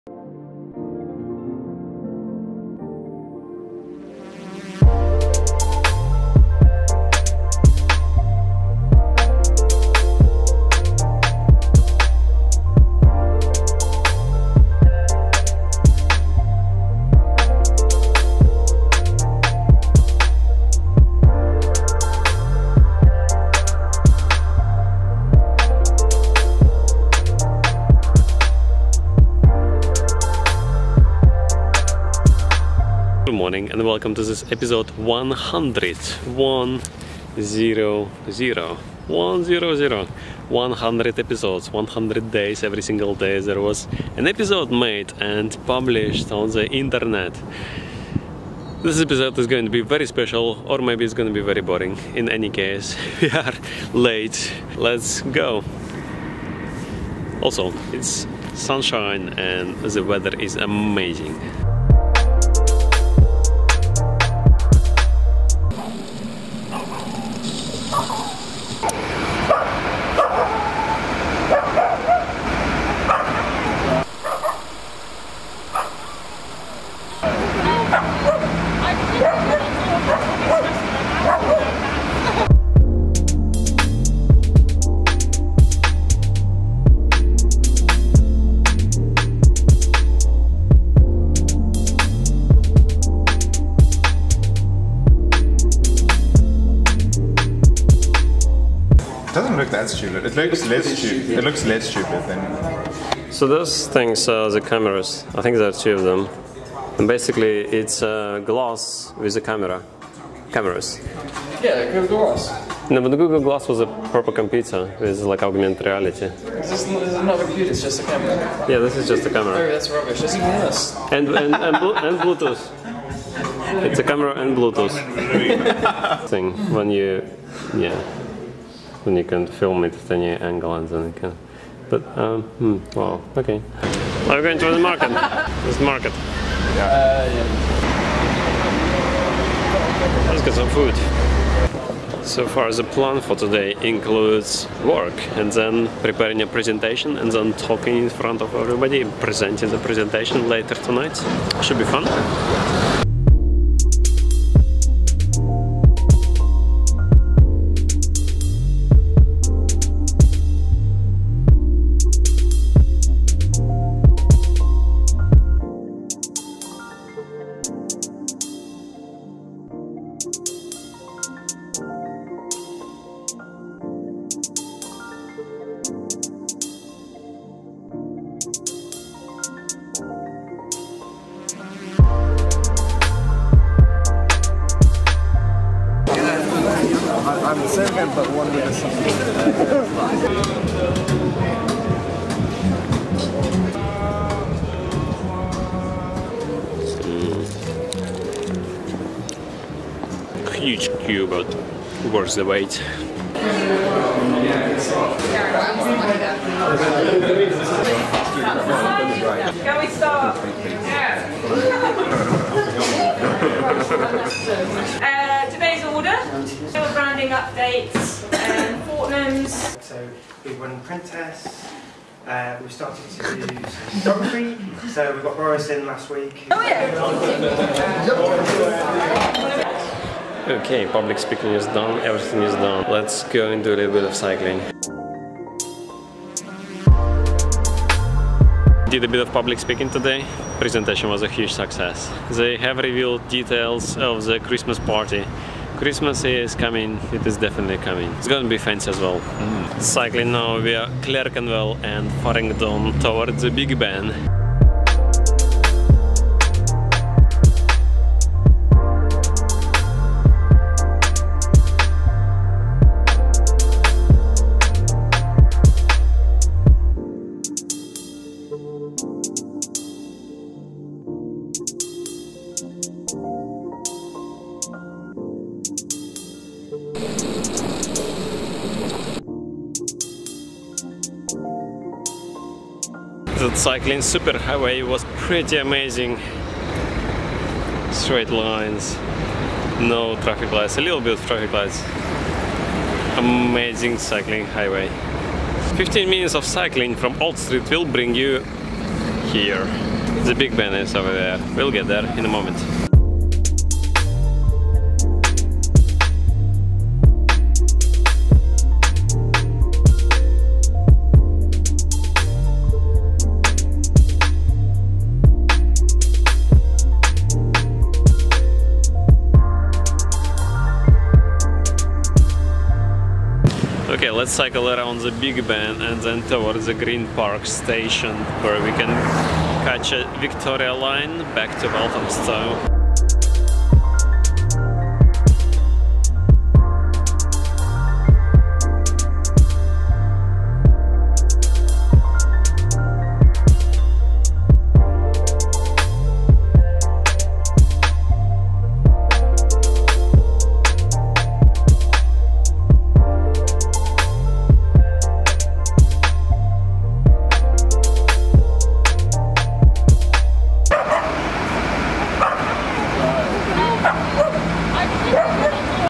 ONE HUNDRED VLOG EPISODES
Alex Savin vlog episode 100

Cycling - trains - LONDON EYE timelapsing - Westminster - Red Badger - dogs - cats - tube - lights